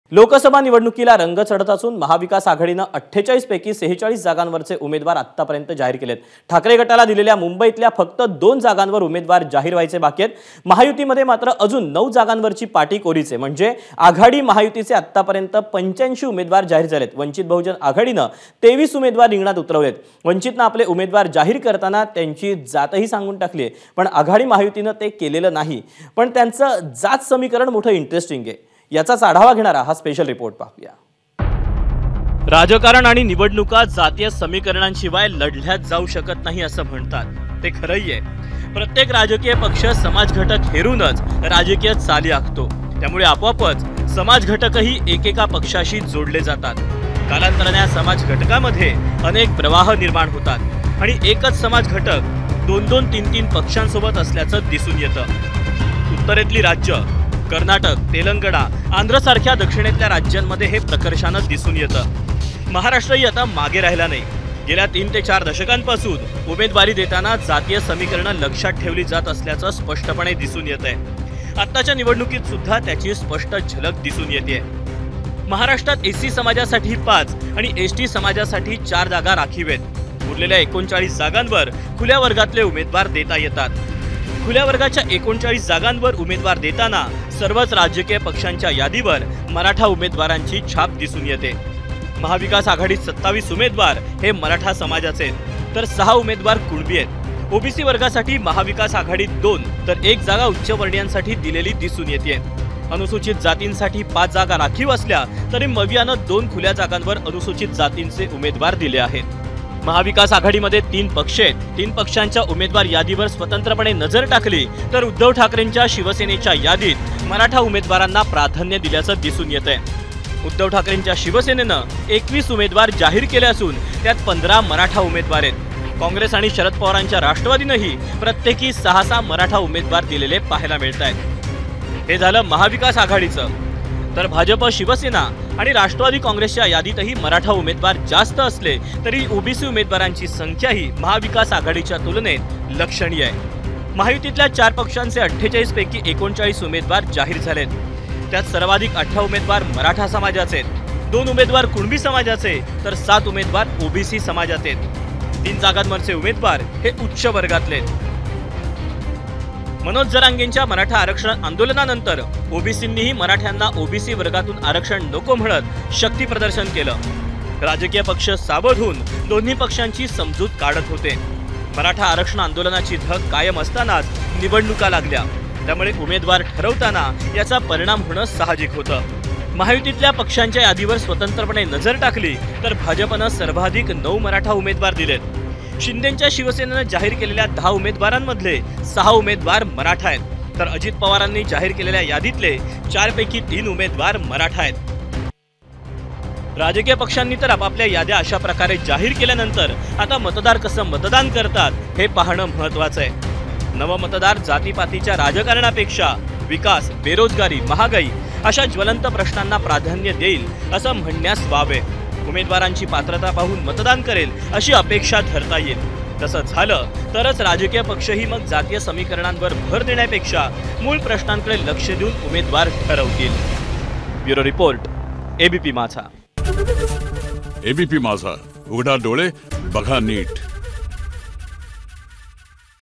Special Report Maharashtra Cast Politics : उमेदवार याद्यांवर जातीय गणित भारी ?
लोकसभा निवडणुकीला रंग चढत असून महाविकास आघाडीनं 48 पैकी 46 जागांवरचे उमेदवार आतापर्यंत जाहीर केलेयत. ठाकरे गटाला दिलेल्या मुंबईतल्या फक्त दोन जागांवर उमेदवार जाहीर व्हायचे बाकी आहेत. महायुतीमध्ये मात्र अजून ९ जागांवरची पाटी कोरीच आहे. म्हणजे आघाडी-महायुतीचे आतापर्यंत ८५ उमेदवार जाहीर झालेयत. वंचित बहुजन आघाडीनंही २३ उमेदवार रिंगणात उतरवले आहेत. वंचितनं आपले उमेदवार जाहीर करताना त्यांची जातही सांगून टाकलीय, पण आघाडी-महायुतीनं ते केलं नाही. पण त्यांचं जातसमीकरण मोठं इंटरेस्टिंग आहे..पाहू या स्पेशल रिपोर्ट...